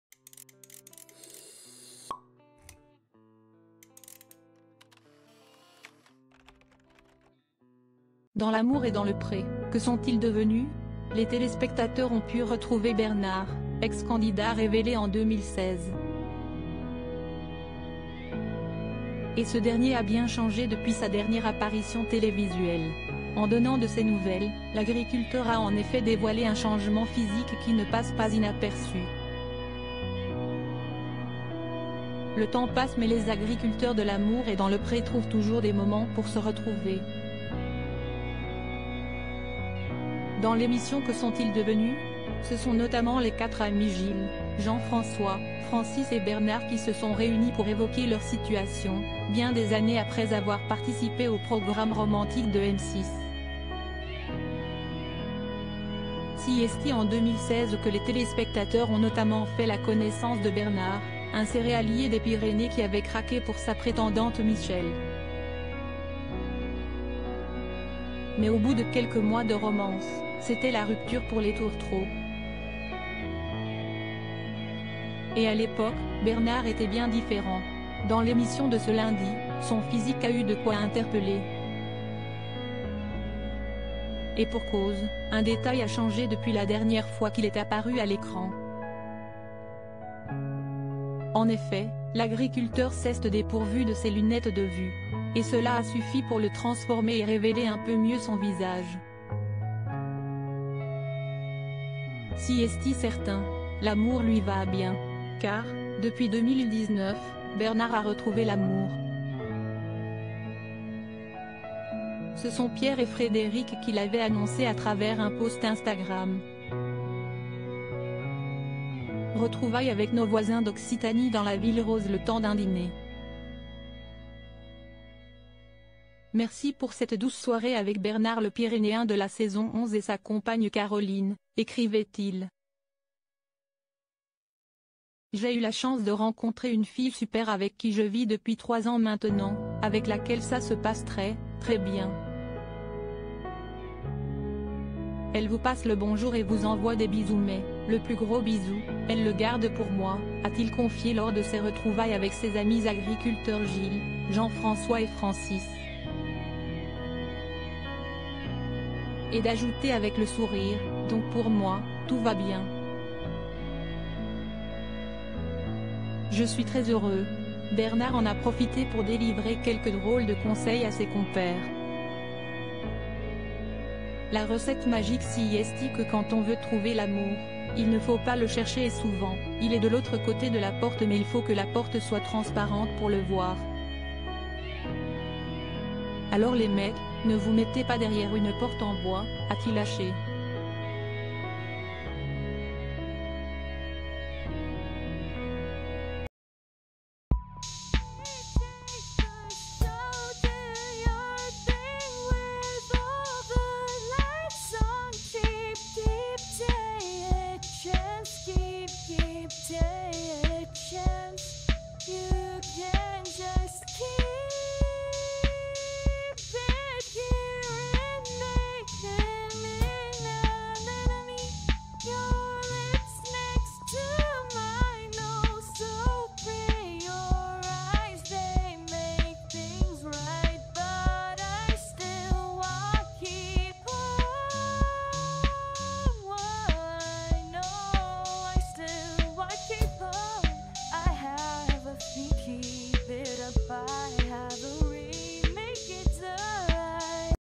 Bernard (L'Amour est dans le pré) heureux en amour et transformé : son changement physique tout part
Bernard (L'Amour est dans le pré) heureux en amour et transformé : son changement physique tout particulier